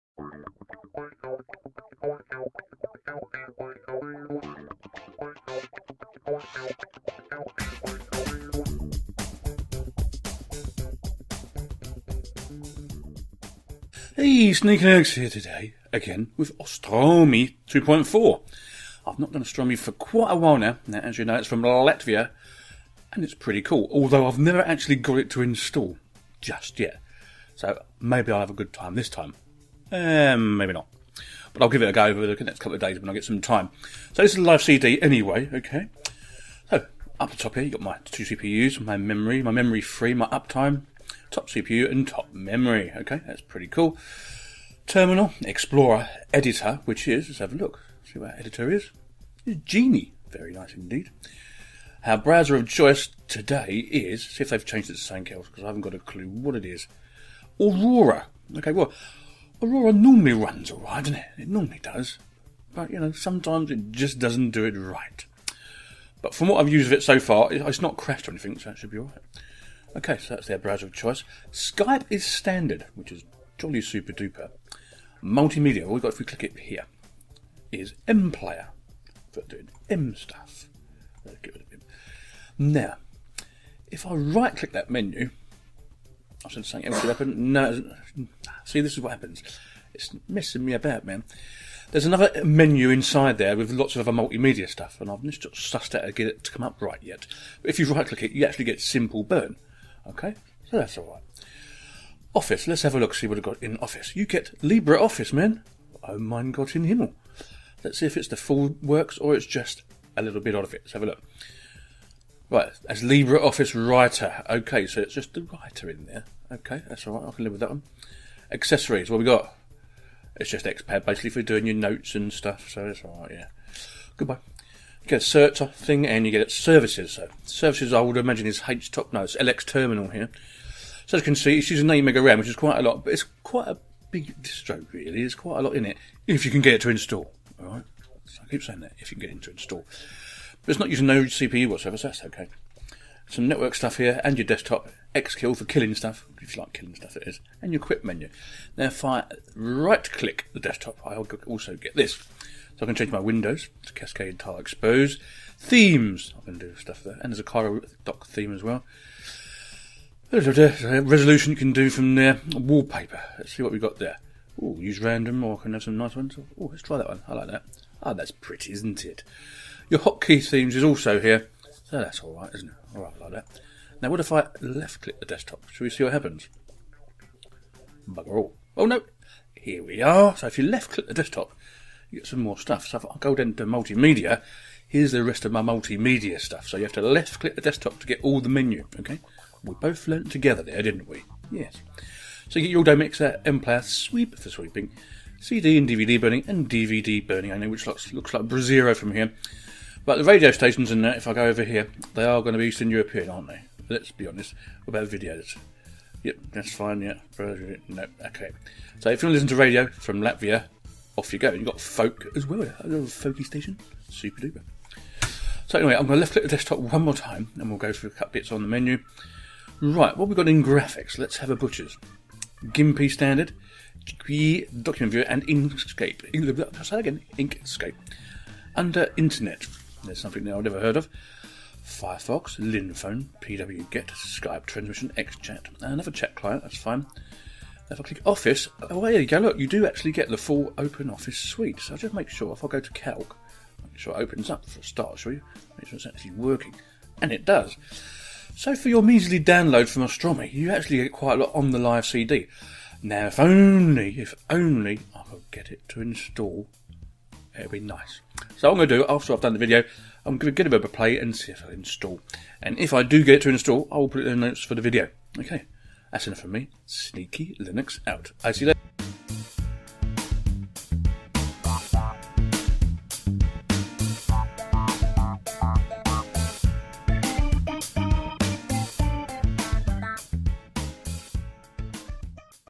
Austrumi 2.4.0.......Latvian Linux
here we take a trip to eastern europe and take a return look at austrumi after about 10 months, now i still have not been able to get it to install just yet, but i will give it a go in a day or so.
it really has everything you need for a system so why not give it a go.